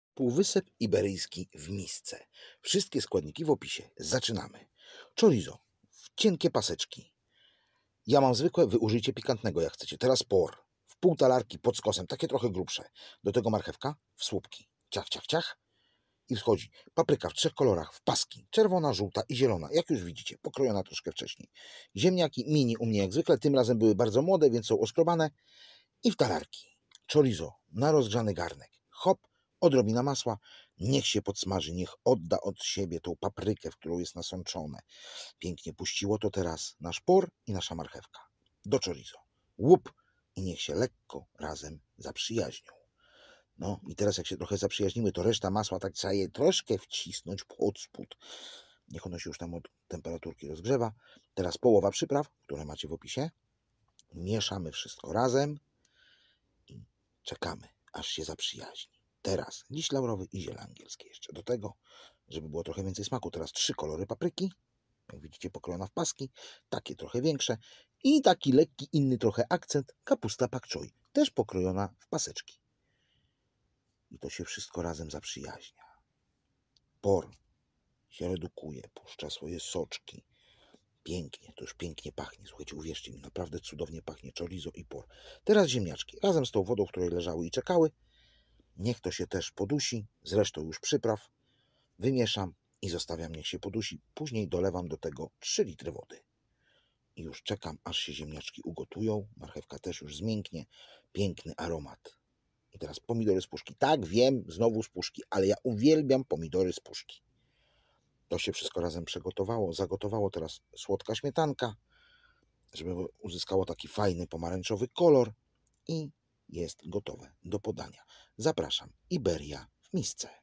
Pyszna Iberia. Delicious Iberia
Pyszna i delikatna zupa z chorizo  Przyprawy: Sól 1 łyżka stołowa, czosnek staropolski 1 łyżeczka  kminek cały 1 łyżeczka, papryka ostra 1łyżeczka  tymianek 1 łyżeczka, gałka muszkatołowa 1 łyżeczka, liść laurowy 4, ziele angielskie kilka kulek( np 5), curry 1 łyżeczka, pieprz czarny 1 łyżeczka (może być świeżo mielony), majeranek 1 łyżka stołowa natka pietruszki do posypania

Składniki: Śmietanka 300 ml, pomidory krojone 1 puszka,   chorizo 200g,  kapusta Pak choi 1, j masło 20g, marchewka 1 sztuka, papryka (pół czerwonej półnuty żółtej i zielonej) port 3 sztuki biała część, ziemniaki młode 300g.

Przyprawy wymieszaj razem z wyjątkiem liścia I ziela (zostaw trochę natki do posypania gotowej zupy)

Chorizo paprykę i marchewkę pokrój w paski, pora przetnij na pół potem pokrój pod skosem na dość grube półksiężyce,  kapustę Pak Choi posiekaj nie zbyt drobne paski. Postaw garnek na palniku wrzuć pokrojone chorizo dodaj 1/3 masła. Kiedy kiełbasa się lekko podsmaży na maśle dodaj pokrojonego pora i marchewkę dokładnie zamieszaj dodaj resztę masła (pomieszaj niech masło znajdzie się na pod warzywami i kiełbasą), przykryj pozostaw na chwilę niech się wszystko poddusi. Teraz dodaj połowę przypraw z miseczki, liść laurowy i ziele angielskie. Dokładnie wymieszaj. Wsyp do garnka paprykę I kapustę. Przykryj niech się podusi przez parę chwil żeby warzywa przeszył przyprawami. Dodaj pokrojone ziemniaki i pozostałe przyprawy. Całość zalej 3 litrami wody. Przykryj gotuj aż warzywa (ziemniaki i marchewka) będą miękkie.  Kiedy tak się stanie wykańczamy zupę pomidorami z puszki, śmietanką i doprawiamy solą i pieprzem. Smacznego

Delicious and delicate chorizo ​​soup  Spices: Salt 1 tablespoon, Old Polish garlic 1 teaspoon, whole cumin 1 teaspoon, hot paprika 1 teaspoon, thyme 1 teasIpoon, nutmeg 1 teaspoon, bay leaf 4, allspice a few balls (e.g. 5), curry 1 teaspoon, black pepper 1 teaspoon (can be freshly ground), marjoram 1 tablespoon, parsley for sprinkling

Ingredients: Cream 300 ml, chopped tomatoes 1 can, chorizo ​​200g, Pak choi cabbage 1, butter 20g, carrot 1 piece, bell pepper (half a red, half a yellow and green) port 3 pieces white part, young potatoes 300g.